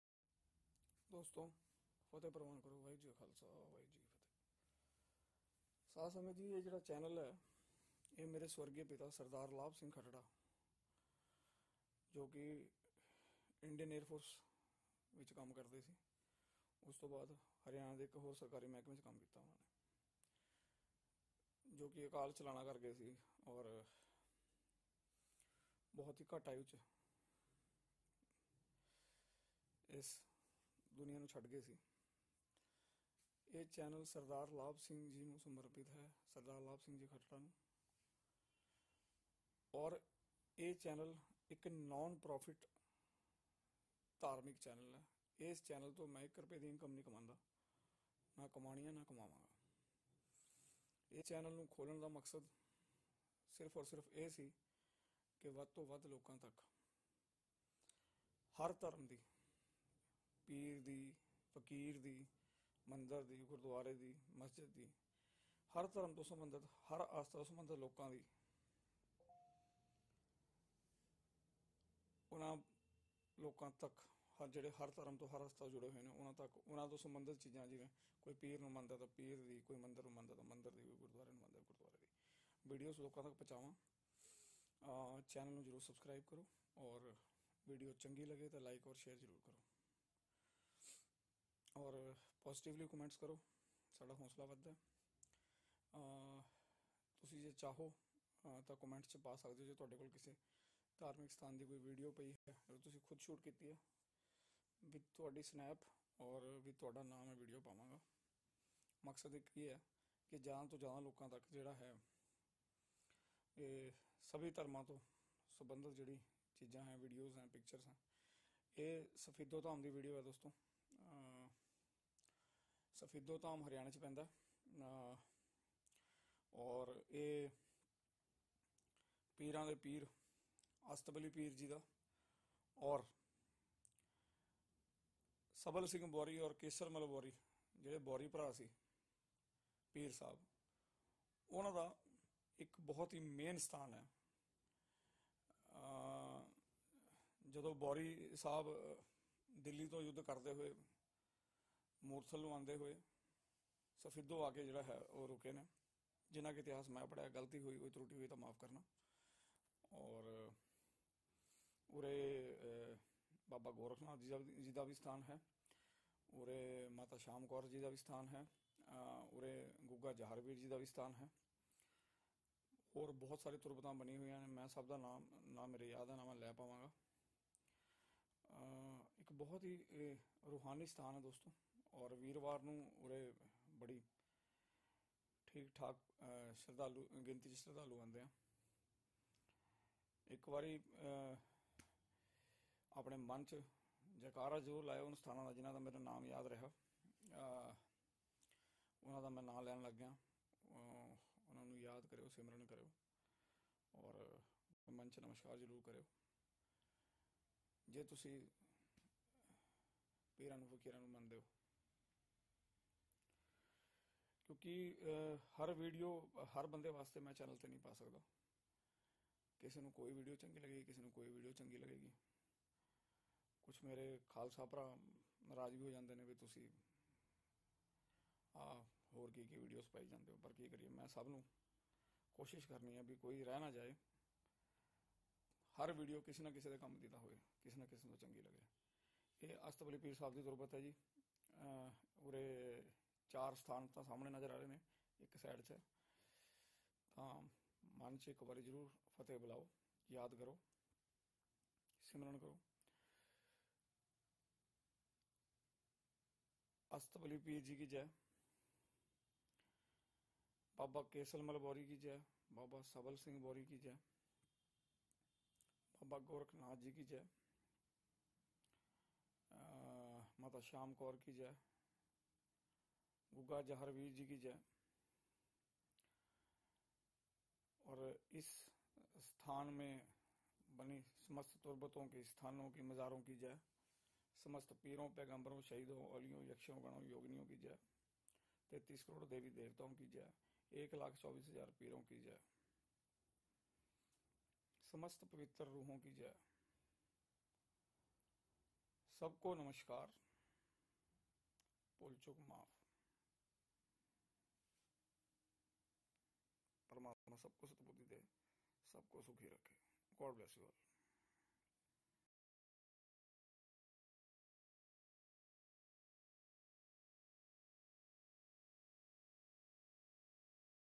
safido dhaam haryana slideshow
astbali peer sahib, keser mal bouri ji ,sabal singh bouri sahib ji ko samrpit safidoh dhaam

yaha video bananey ki ijajat nahi mili ji isliye slideshow banaya ji